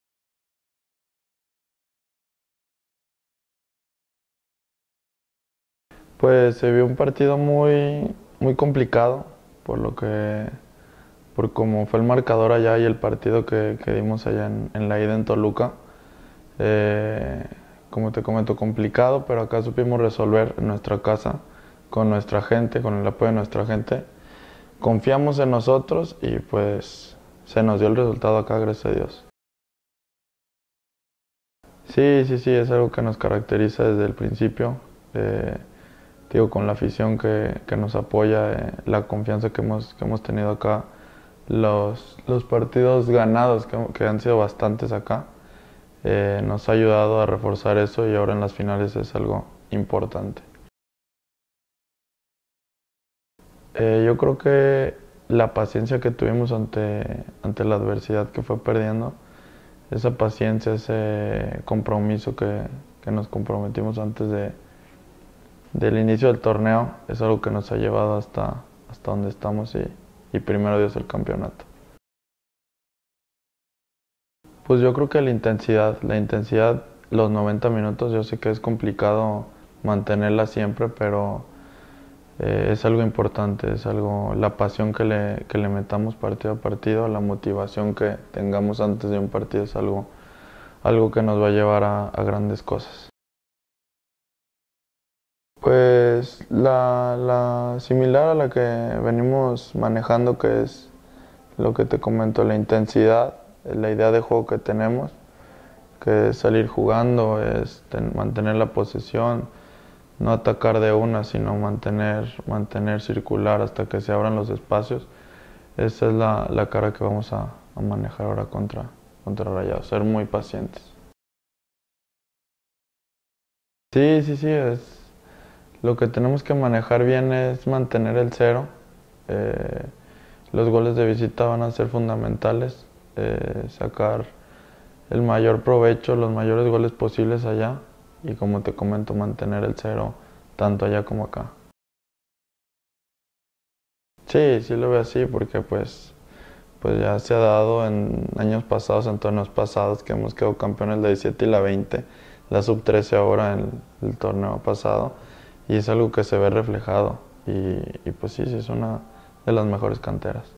Entrevista: Joel García - Portero Santos Sub 20
En entrevista con Joel García, nuestro guardameta del conjunto Sub 20 habló sobre el sentir del equipo tras avanzar a semifinales del Torneo Clausura 2019.